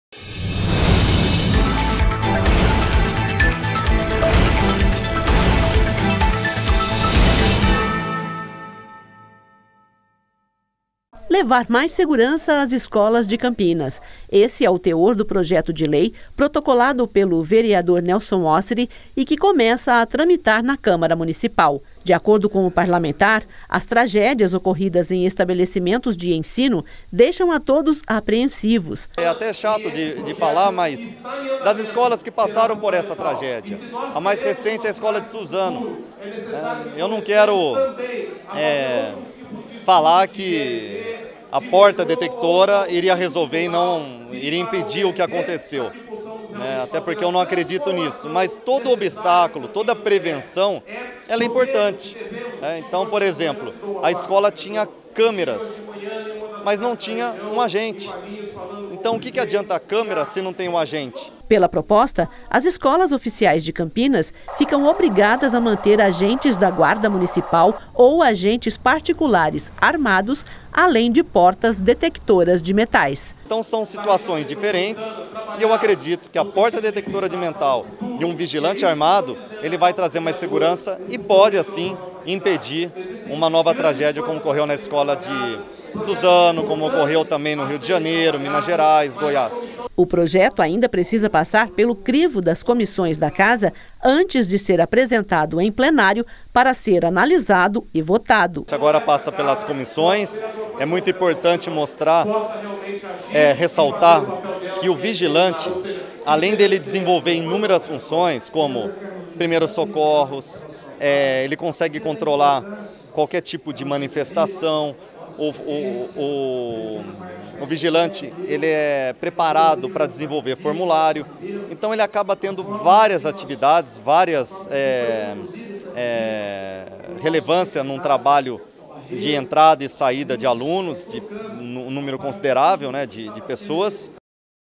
Câmara Notícia : Segurança nas escolas 27/03/2019
O vereador Nelson Hossri (PODEMOS) protocolou um projeto de lei (PLO 65/19) que obriga escolas de Campinas a manterem um agente armado e portas detectoras de metal.